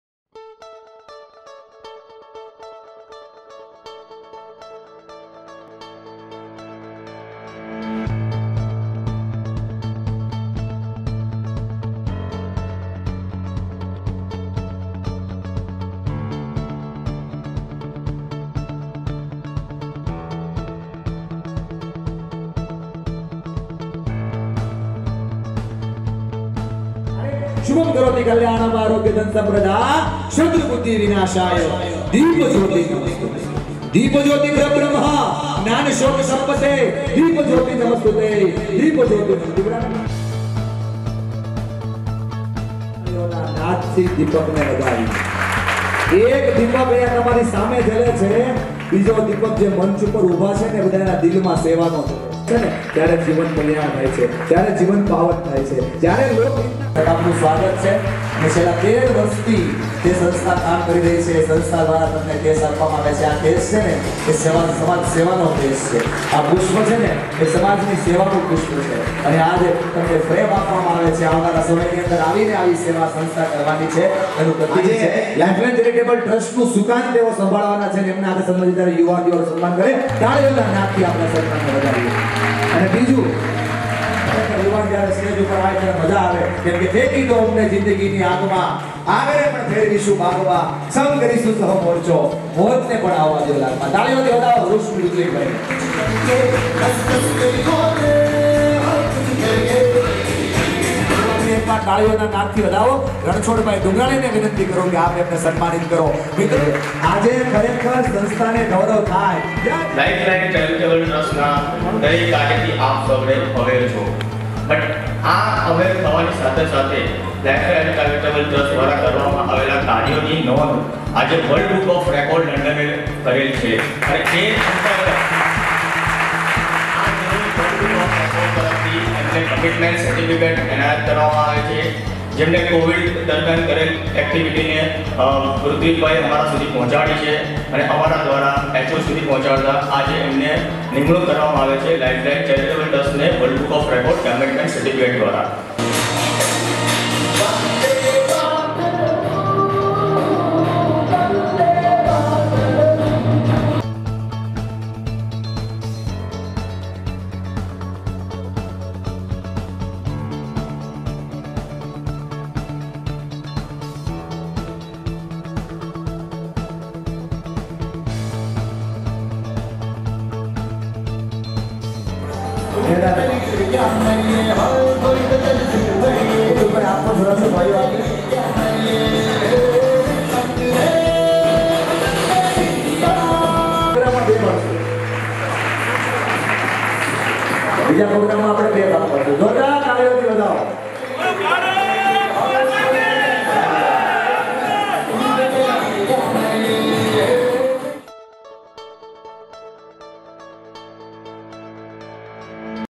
LIFE LINE UNITED FOUNDATION
લંડન સ્થિત વર્લ્ડ બુક ઓફ રેકોર્ડ એ સુરતની સેવાકીય સંસ્થા લાઈફ લાઈન ચેરીટેબલ ટ્રસ્ટની નોંધ લીધી સર્ટિફિકેટ એનાયત કરવામાં આવ્યું